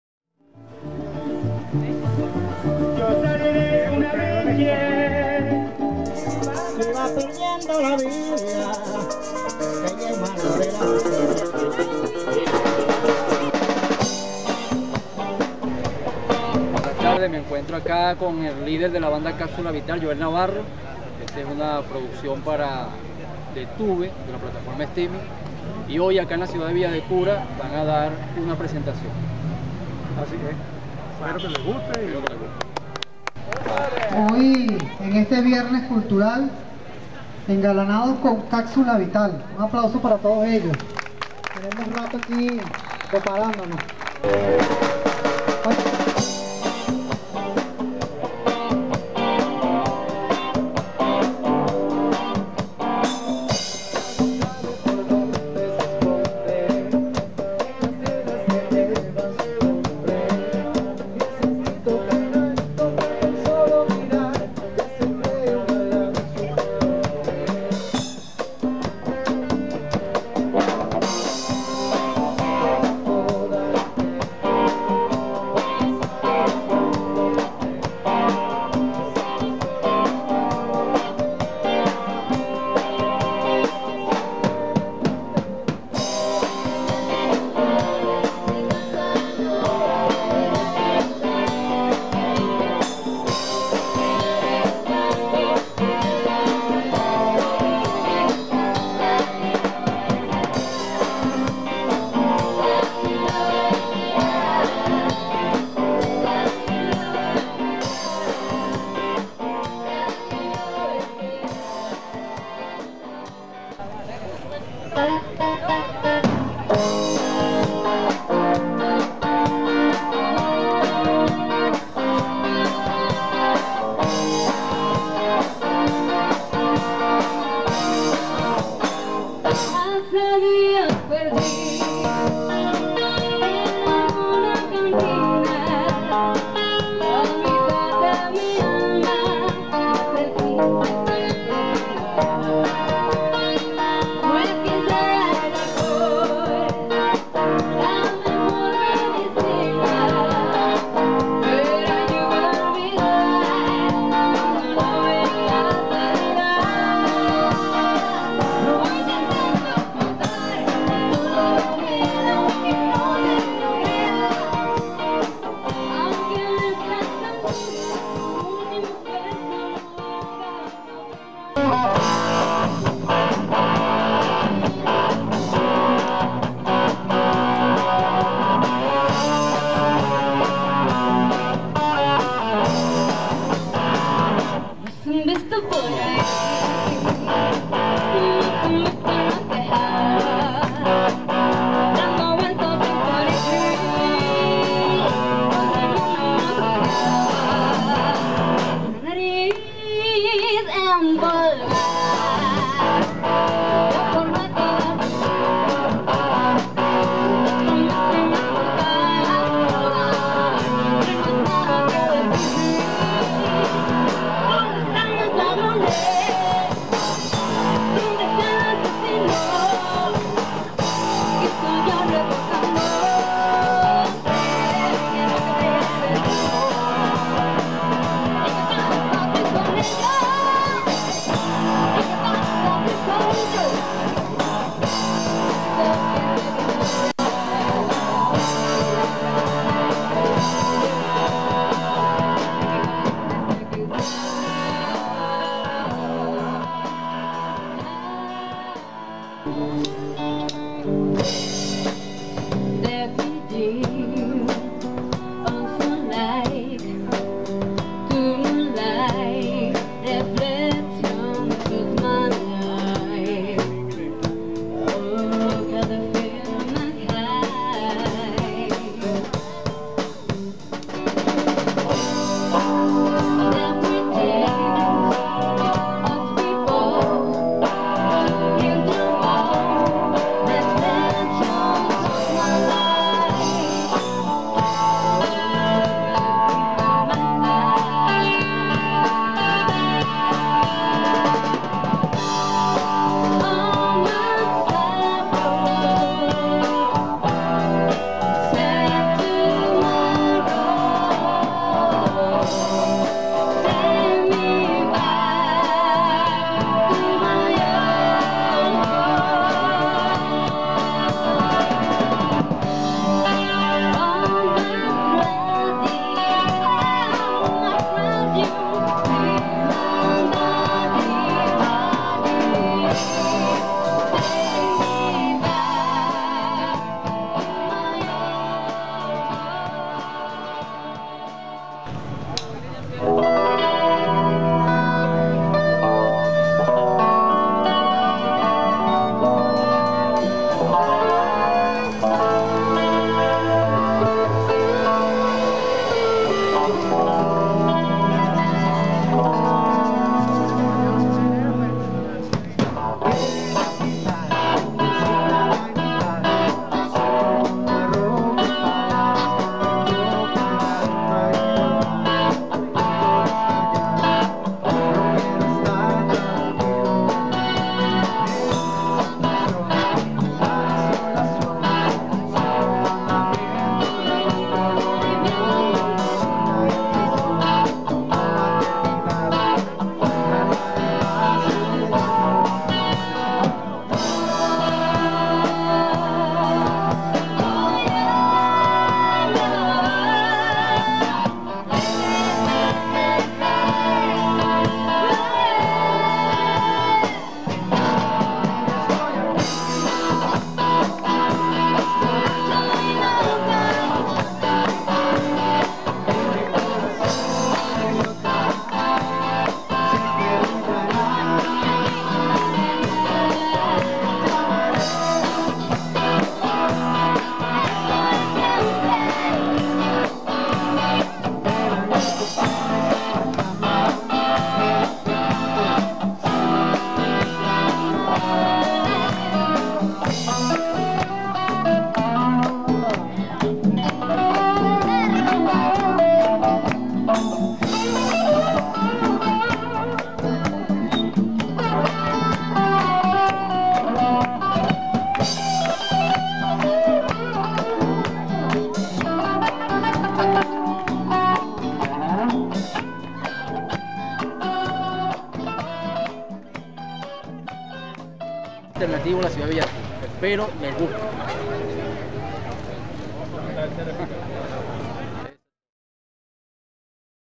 Kpsula Vital - Viernes culturales 13/09/2019 ( resumen de su presentación )
Como una iniciativa de la dirección de cultura del Mcpio. Zamora de la ciudad de Villa de Cura cada día viernes se realizan actividades en la plaza Miranda de dicha ciudad.

Esta vez correspondió el turno a la Banda de Rock Alternativo Kpsula Vital del profesor Joel Navarro y sus músicos quienes deleitaron al público presente con grandes canciones al mejor estilo retro en español e ingles. En este video observamos a la vocalista principal de la banda, la srita. Iraly Mejías interpretando junto a Henrry  grandes canciones del pop y el rock de las décadas de los 70 - 80 y 90

Esta es un video original de Gaborockstar producciones para Youtube y Dtube en la plataforma steemit. Villa de Cura viernes 13 de Sept. de 2019.